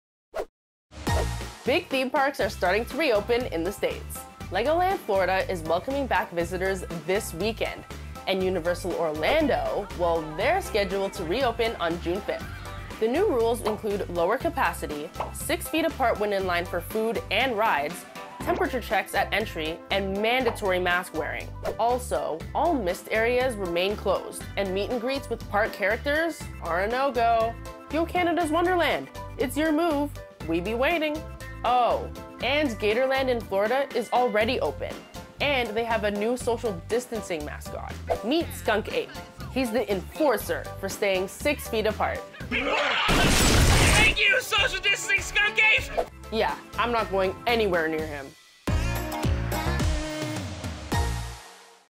Florida Theme Parks Opening! Legoland, Universal Studios, Gatorland & their New Normal | CBC RECAP
Florida theme parks are back! Legoland, Universal Studios, Gatorland & their new normal | CBC RECAP

Updates on theme parks in Florida. Legoland is opening, Universal Studios is opening, Gatorland is already open and Walt Disney World is reopening soon too! Is Canada's Wonderland next?

CBC RECAP is powered by CBC Kids News! Myah Elliott and the squad share a weekly RECAP of buzzed-about news and pop culture stories for teens.